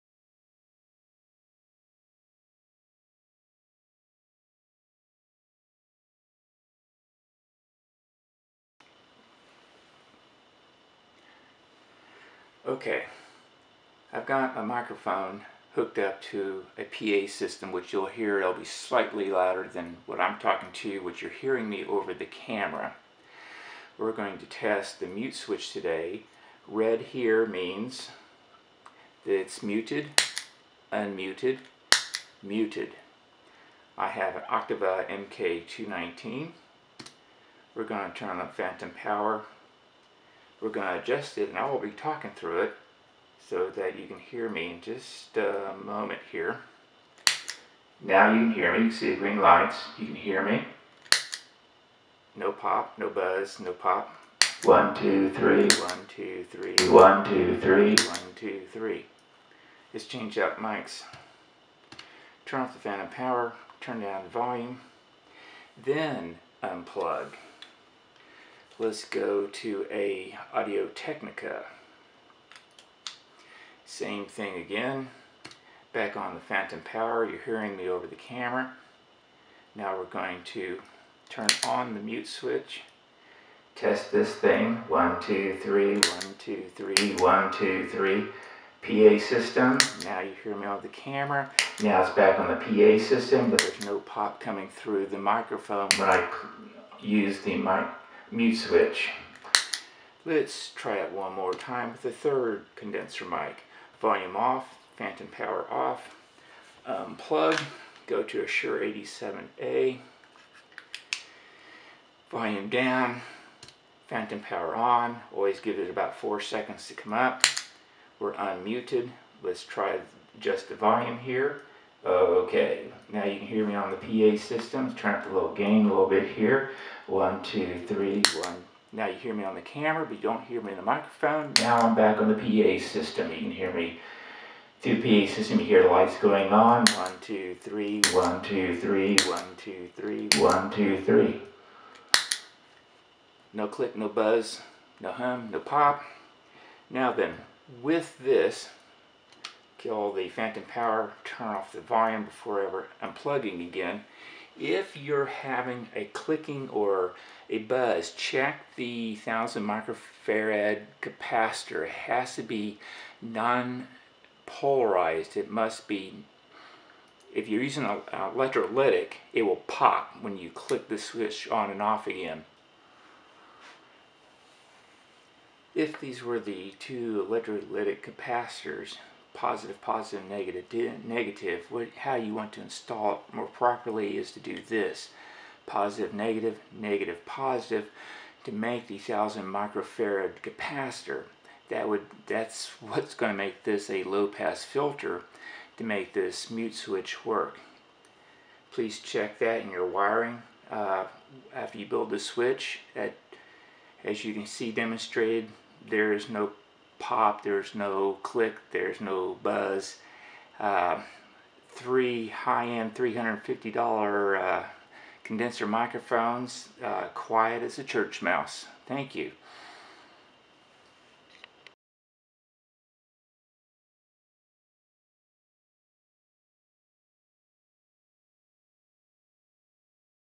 Testing Mute Switch with Condenser Microphones
Some viewers have said that the mute switch have a buzz, fuzz or click when using a condenser switch. It may very well be that the 1000uF capacitor is polarized and it is not suppose to be. Also, check that your 48V phantom power does not drop below 44 volts, because that causes the microphone to have issues beyond the mute switch.  So, I tested 3 common condenser mics and posted this video to demonstrate that the mute switch does work. Now then, the ABY video will not work with a condenser mic UNLESS you supply phantom power to the INPUT side of the mute switch. This is because the output side disconnects pin 2 from the output jack.  If you need a phantom power supply get a Sterling PHP1 for $40, hook the mic to the PHP1 and then connect to the input side of the mute or ABY mute switch. You'll need one for each mic.